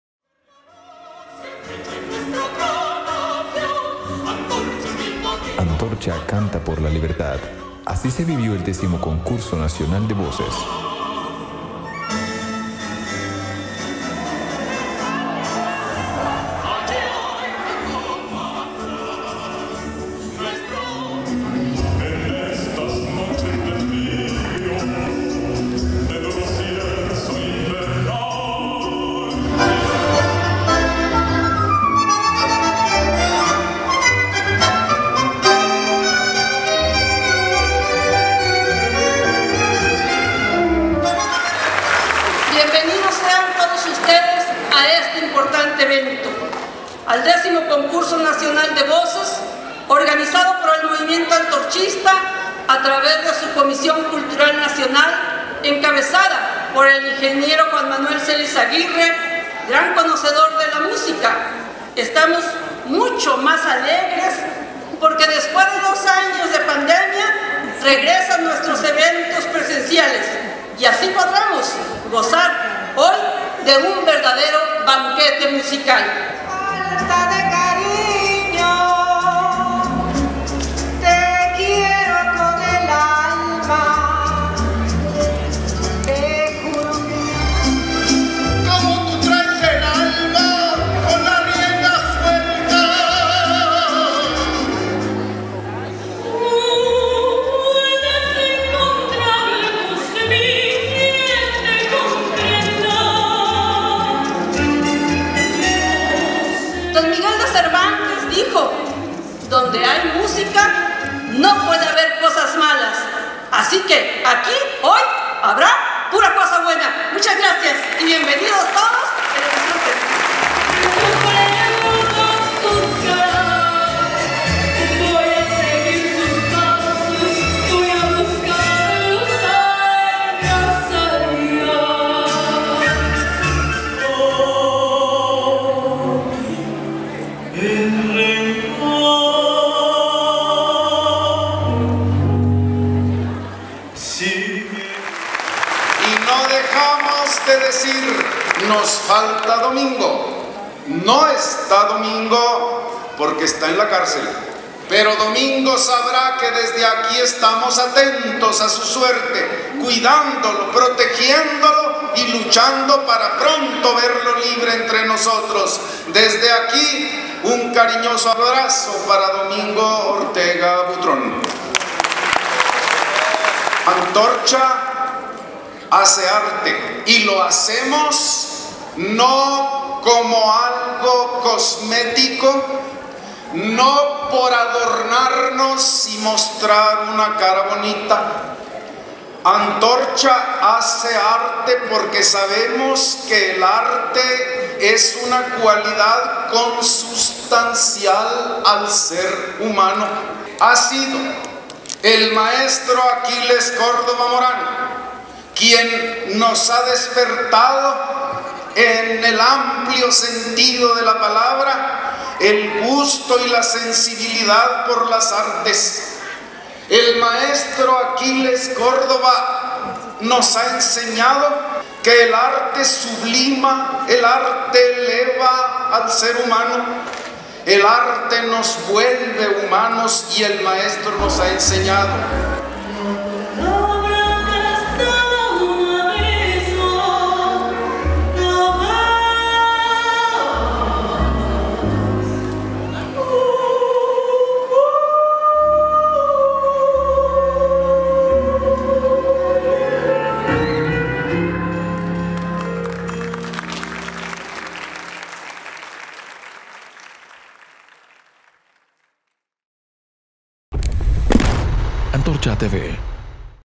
X Concurso Nacional de Voces
Para el antorchismo es de suma importancia que el pueblo conozca y haga arte, pues es una herramienta de sensibilización ante las injusticias que sufre diariamente; en este sentido, la labor de Antorcha por acercar el arte a las clases con más carencias ha llevado a la realización de eventos culturales nacionales donde el pueblo es el gran protagonista, como el X Concurso Nacional de Voces.